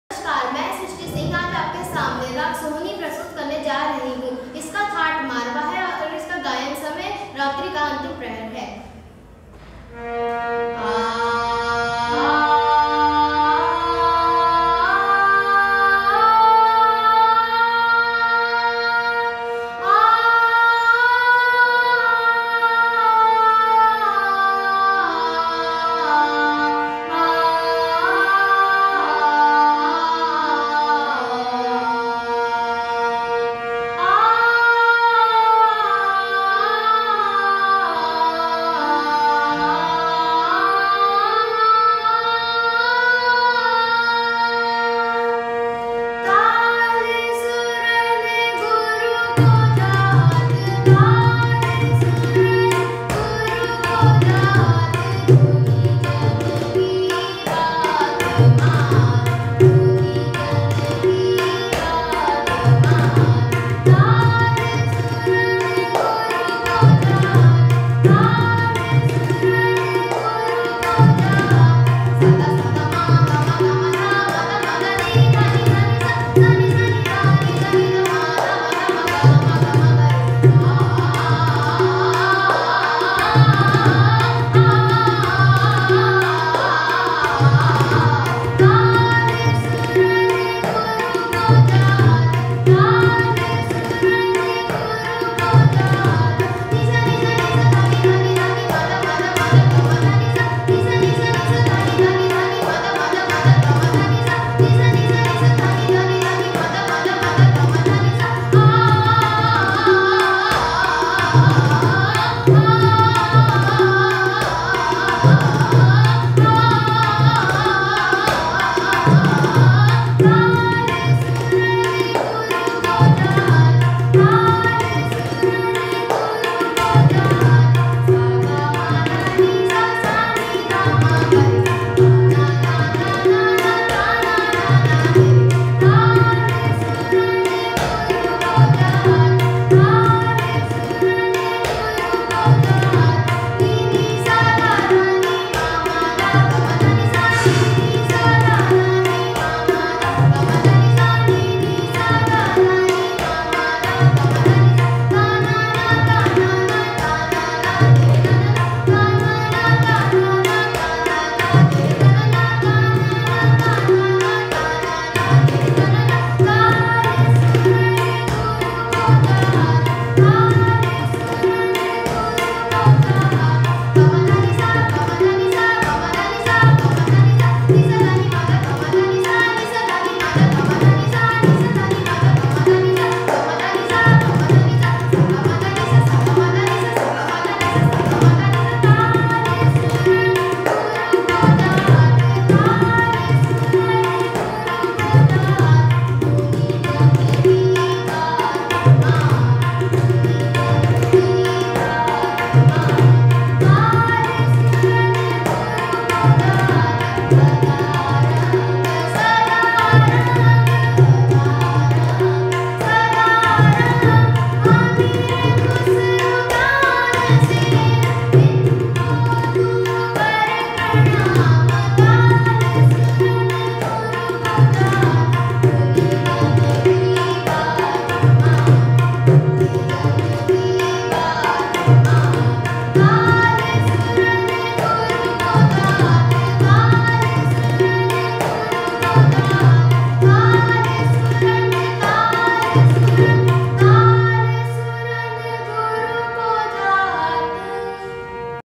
VOCAL CLASSICAL (KALAUTSAV) 2021-22
School - SR Capital Public School
School ID - 1105190
Category Junior
Raag - Sohini
Srishti Singh
Class 10